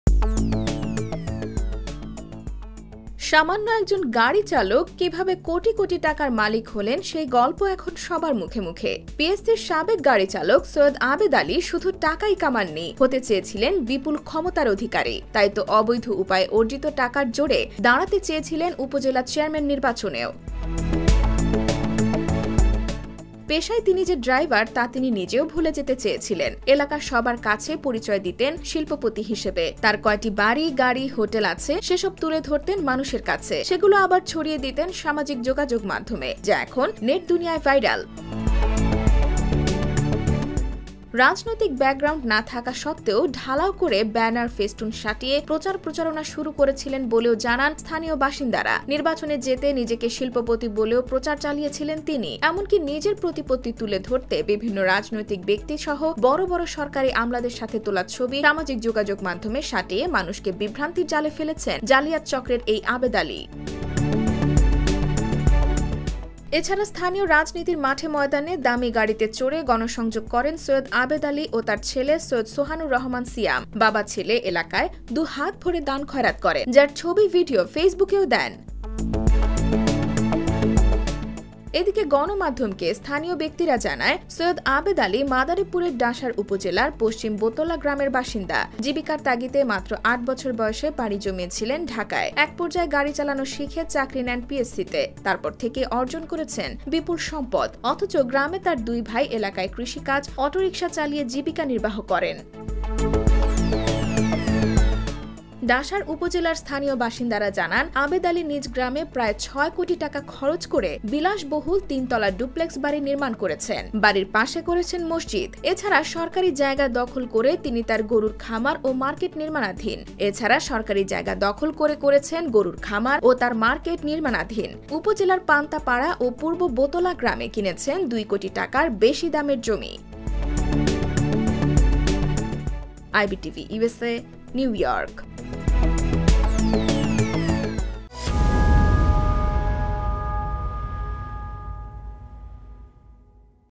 শিল্পপতির পরিচয়ে উপজেলা চেয়ারম্যান নির্বাচনে নেমেছিল এই ড্রাইভার | Upazila Election | Identity Swap
সামন্য একজন গাড়িচালক কিভাবে কোটি কোটির টাকার মালিক হলেন সেই গল্পে এখন সবার মুখে মুখে। পিএসসির সাবেক গাড়িচালক সৈয়দ আবেদ আলীও শুধু টাকাই কামাননি, হতে চেয়েছিলেন বিপুল ক্ষমতার অধিকারী। তাইতো অবৈধ উপায়ে অর্জিত টাকার জোরে দাড়াতে চেয়েছিলেন উপজেলা চেয়ারম্যান নির্বাচনেও।  পেশায় তিনি যে ড্রাইভার তা তিনি নিজেও ভুলে যেতে চেয়েছিলেন। এলাকার সবার কাছে পরিচয় দিতেন শিল্পপতি হিসেবে। তার কয়টি বাড়ি, গাড়ি, হোটলে আছে সেসব তুলে ধরতেন মানুষের কাছে। সেগুলো আবার ছড়িয়ে দিতেন সামাজিক যোগাযোগ মাধ্যমে। যা এখন নেট দুনিয়ায় ভাইরাল।